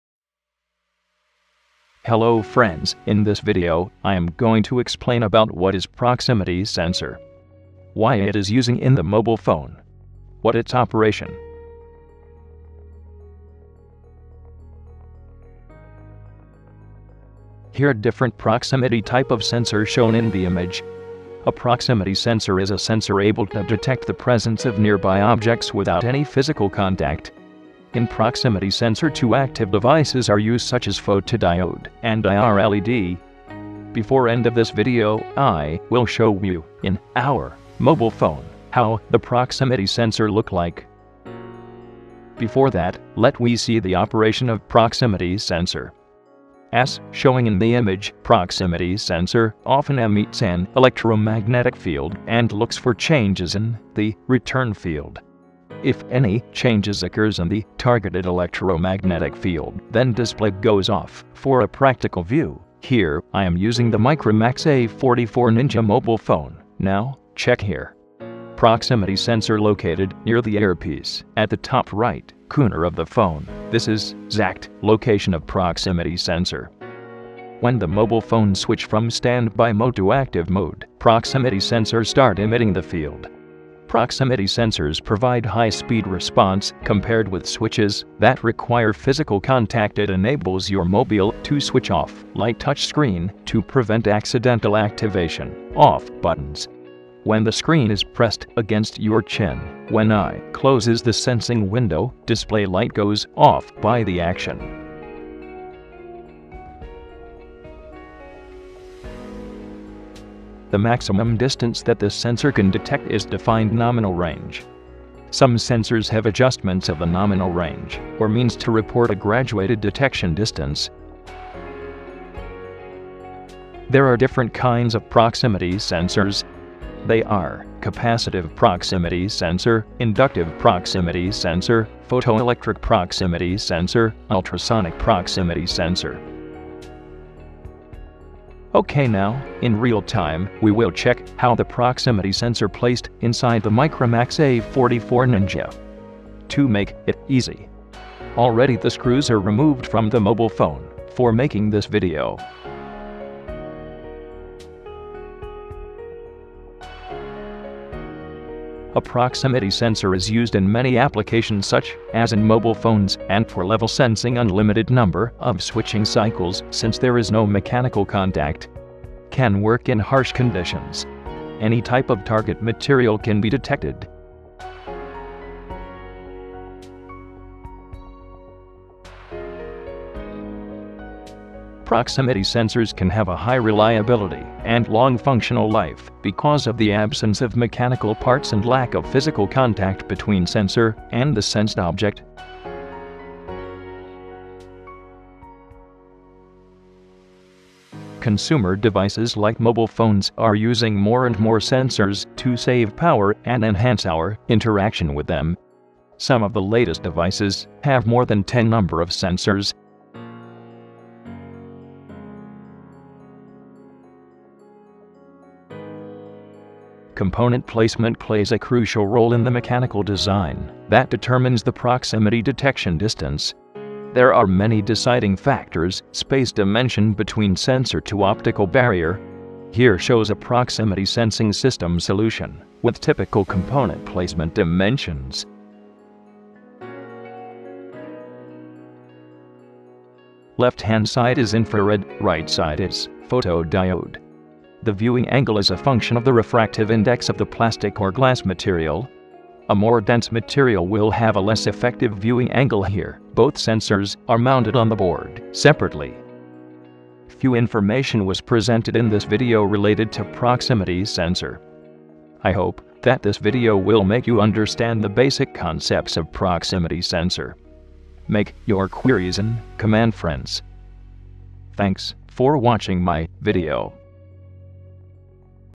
Proximity Sensor in Mobile Phones
A proximity sensor is a sensor able to detect the presence of nearby objects without any physical contact.
A proximity sensor often emits an electromagnetic field or a beam of electromagnetic radiation (infrared, for instance), and looks for changes in the field or return signal. The object being sensed is often referred to as the proximity sensor's target. Different proximity sensor targets demand different sensors. For example, a capacitive photoelectric sensor might be suitable for a plastic target; an inductive proximity sensor always requires a metal target.
The maximum distance that this sensor can detect is defined "nominal range". Some sensors have adjustments of the nominal range or means to report a graduated detection distance.
Proximity sensors can have a high reliability and long functional life because of the absence of mechanical parts and lack of physical contact between sensor and the sensed object.
Proximity sensors are also used in machine vibration monitoring to measure the variation in distance between a shaft and its support bearing

For any quires make a command frnds